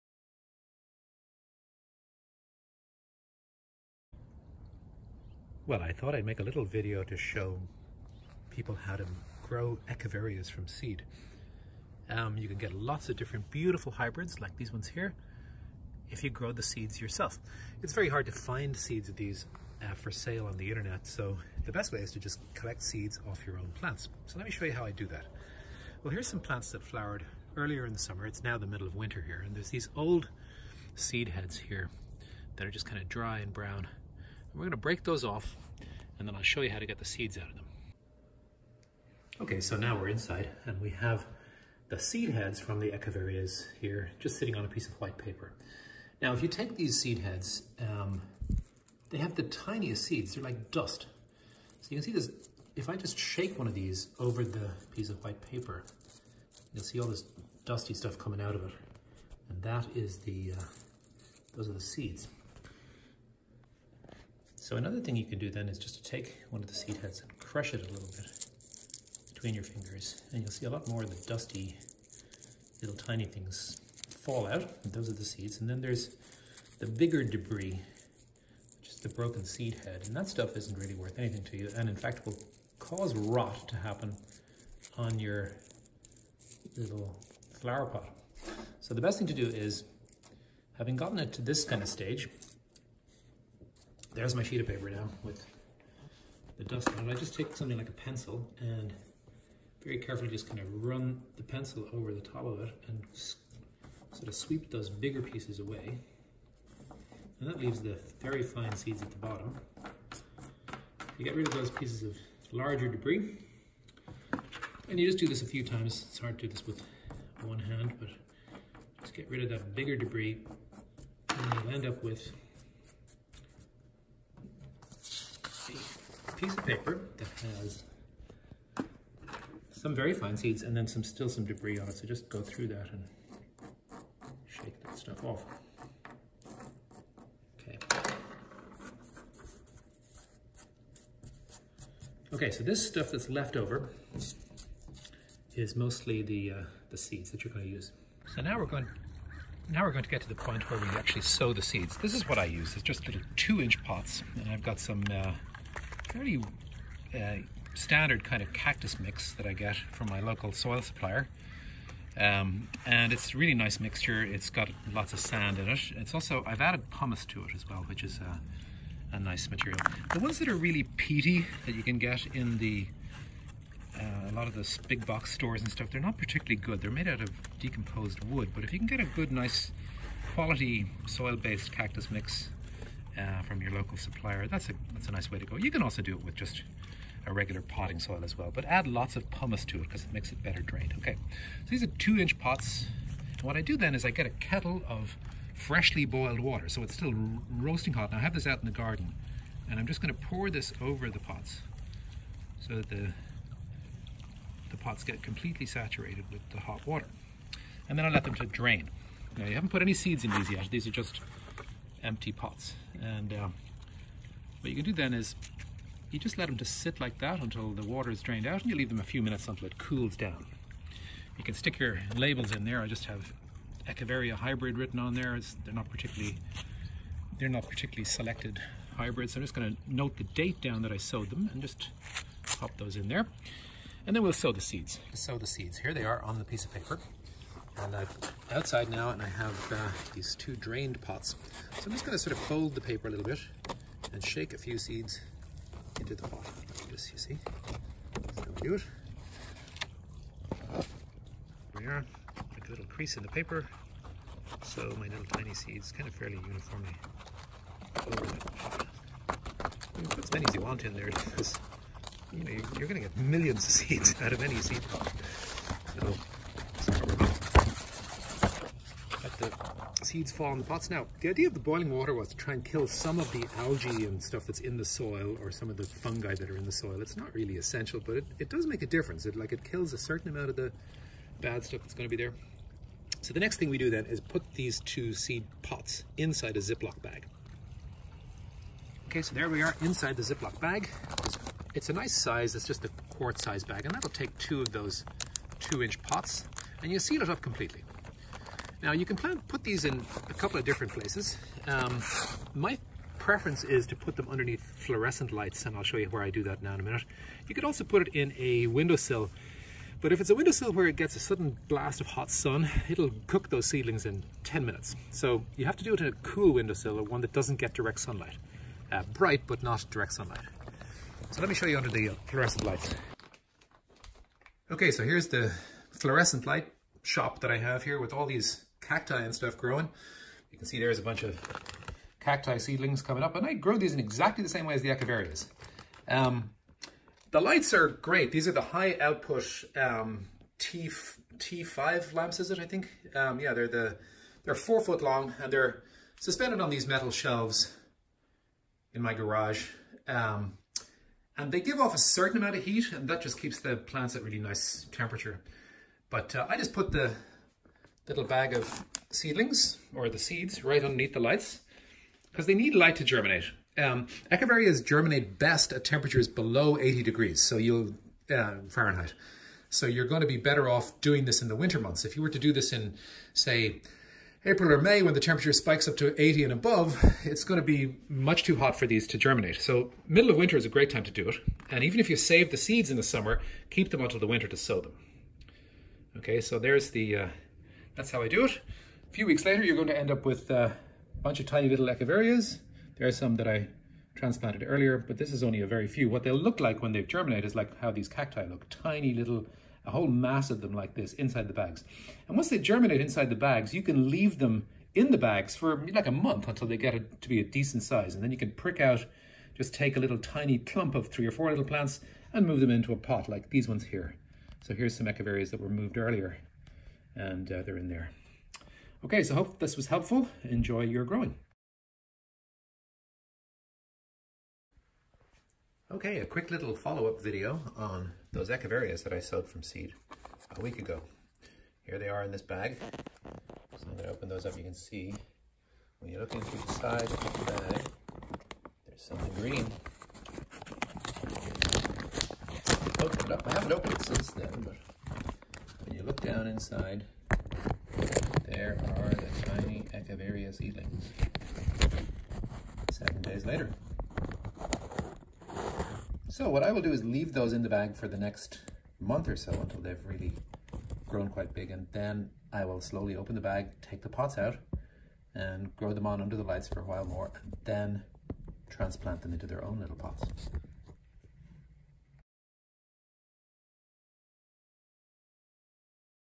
Echeverias from seed
A video explaining how to grow echeverias from seed